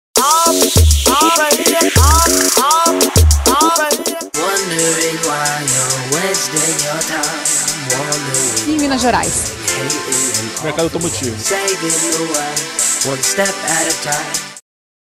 Evento 2 | Lexus, divisão de luxo Toyota em BH
Inauguração do espaço Lexus, marca que é divisão de luxo da Toyota, em Belo Horizonte. Imagens gravadas na concessionária Osaka BH.